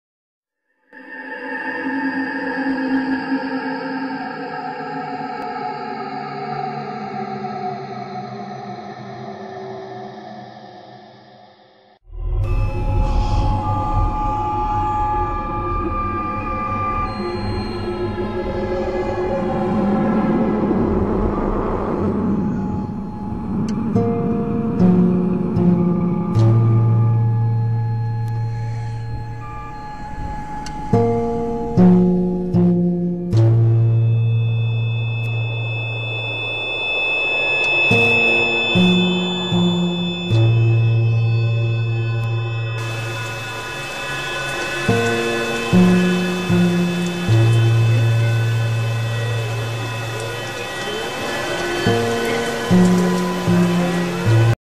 Bratz: Don't Be Afraid of the Dark *TRAILER*
This is gonna be awesome!:D BTW this is by me and my friend 13smexiichick so all the episodes will be uploaded on hers and my accounts!